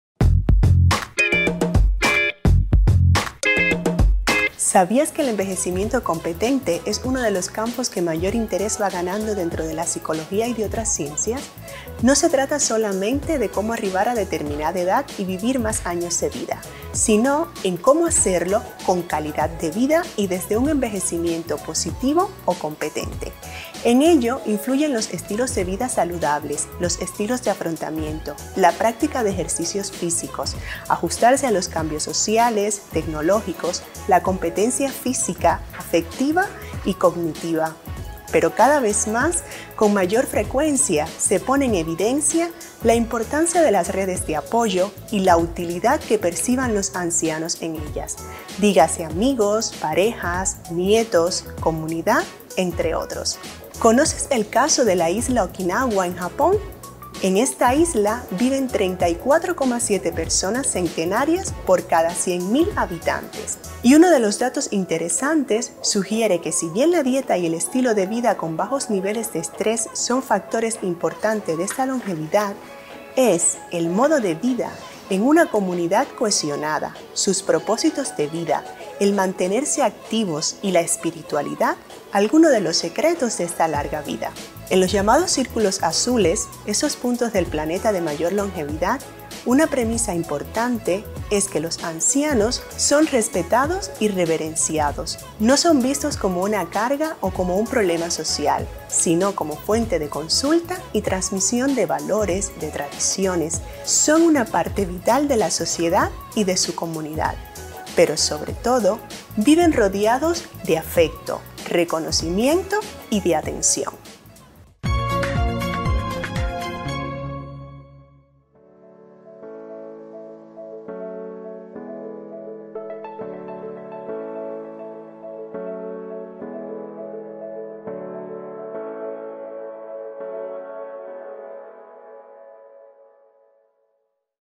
¿SabíasQue​...? Envejecimiento competente
¿Te apasiona la psicología? ¡Infórmate sobre el Grado en Psicología de la Universidad Isabel I!

Mirtha del Prado, profesora del Posgrado en Psicología Clínica y de la Salud, nos cuenta que el envejecimiento competente es uno de los campos que mayor interés va ganando dentro de la Psicología y de otras ciencias.

No se trata solamente de cómo arribar a determinada edad y vivir más años de vida, sino en cómo hacerlo con calidad de vida y desde un envejecimiento positivo o competente.

En ello influyen los estilos de vida saludables, los estilos de afrontamiento, la práctica de ejercicios físicos, ajustarse a los cambios sociales, tecnológicos, la competencia física, afectiva y cognitiva, pero cada vez con mayor frecuencia se pone en evidencia la importancia de las redes de apoyo y la utilidad que perciban los ancianos en ellas: dígase amigos, parejas, nietos, comunidad, entre otros.

¿Conoces el caso de la isla Okinawa en Japón? En esta isla viven 34,7 personas centenarias por cada 100 000 habitantes. Y uno de los datos interesantes sugiere que si bien la dieta y el estilo de vida con bajos niveles de estrés son factores importantes de esta longetividad, es el modo de vida en una comunidad cohesionada, sus propósitos de vida, el mantenerse activos y la espiritualidad, algunos de los secretos de esta larga vida.

En los llamados círculos azules, esos puntos del planeta de mayor longevidad, una premisa importante es que los ancianos son respetados y reverenciados. No son vistos como una carga o como un problema social, sino como fuente de consulta y transmisión de valores, de tradiciones. Son una parte vital de la sociedad y de su comunidad. Pero, sobre todo, viven rodeados de afecto, reconocimiento y de atención.